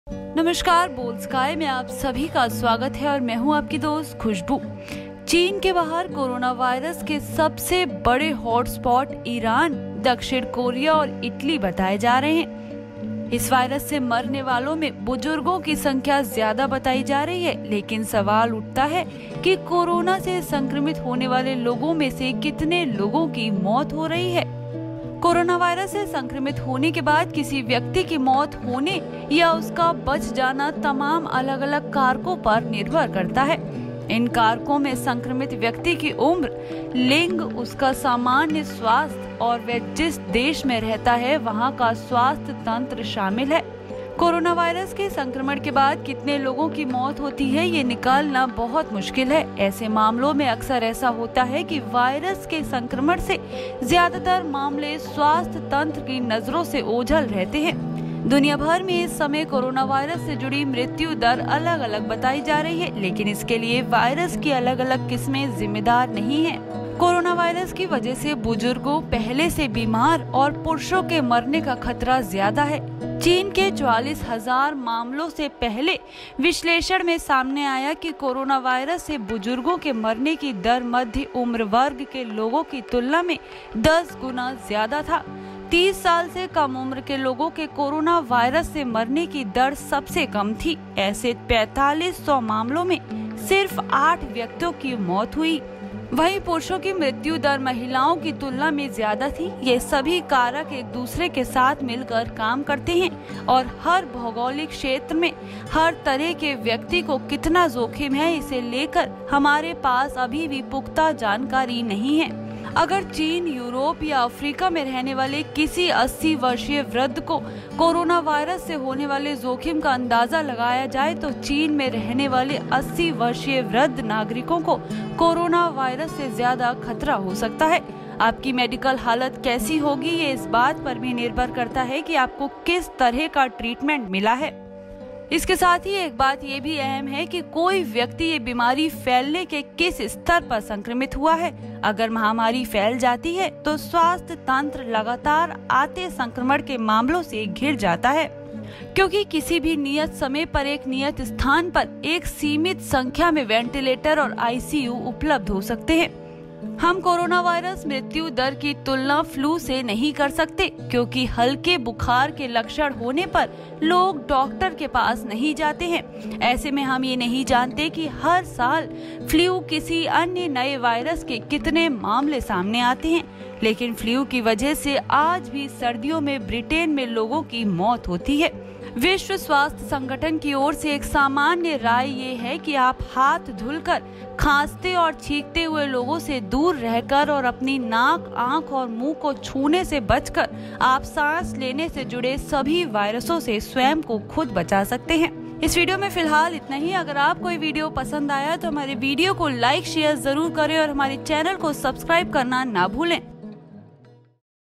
Coronavirus : संक्रमण के बाद बचने की कितनी Possibility है | Boldsky
The largest hot-spots of Coronavirus outside China are being reported as Iran, South Korea and Italy. The number of elderly people dying from this virus is being reported. But the question arises that how many people are dying from corona infected people.

चीन के बाहर कोरोनावायरस के सबसे बड़े हॉट-स्पॉट ईरान, दक्षिण कोरिया और इटली बताए जा रहे हैं.इस वायरस से मरने वालों में बुजुर्गों की संख्या ज़्यादा बताई जा रही है. लेकिन सवाल उठता है कि कोरोना से संक्रमित होने वाले लोगों में से कितने लोगों की मौत हो रही है |